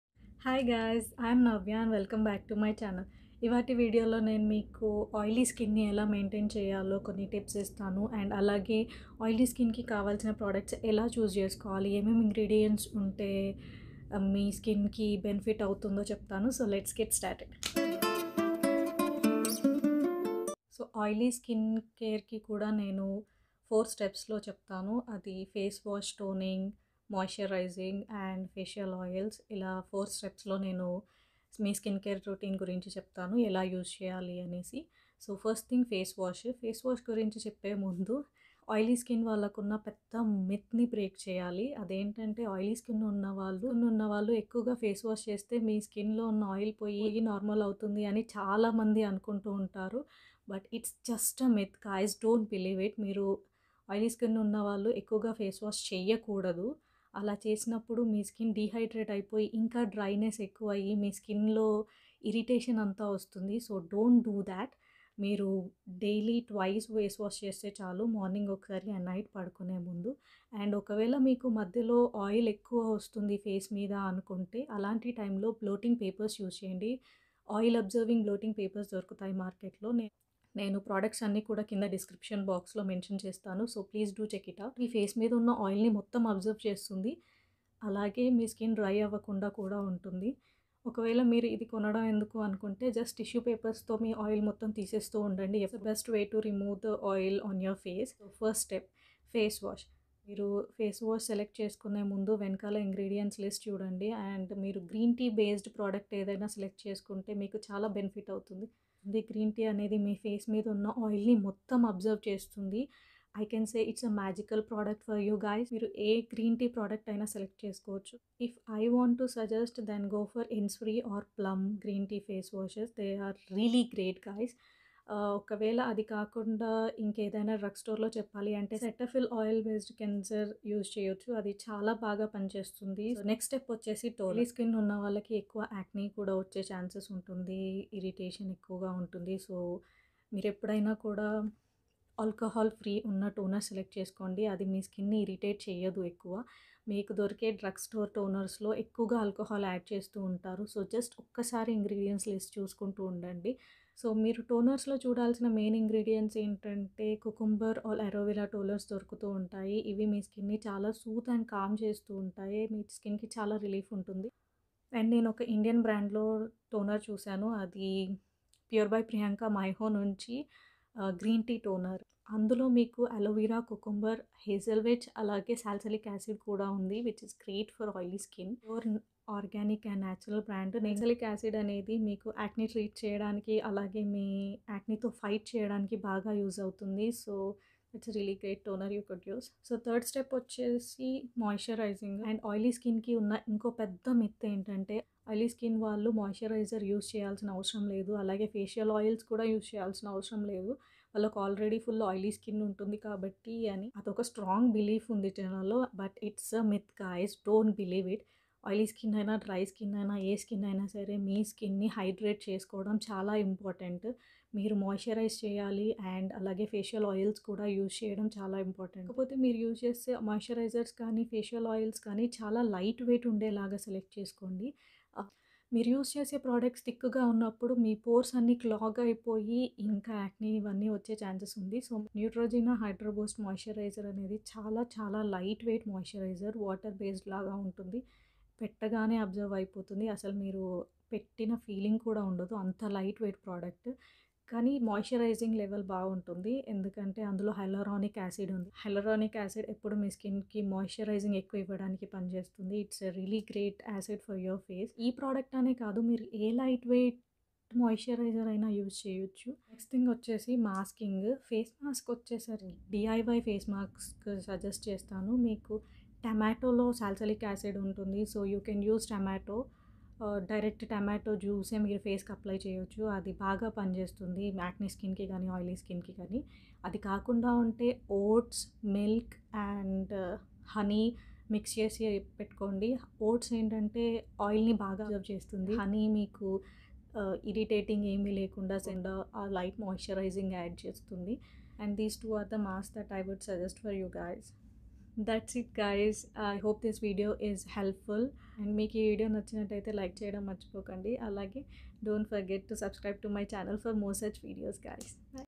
SKIN CARE TIPS FOR -OILY SKIN
my previous videos:
PRODUCTS MENTIONED:
Plum green tea pore cleansing face wash
innisfree green tea hydrating cleansing foam
cetaphil normal to oily skin daily facial cleanser
pure by priyanka myoho(Indian brand not available in usa) green tea toner
neutrogena hydro boost moisturizer for oily skin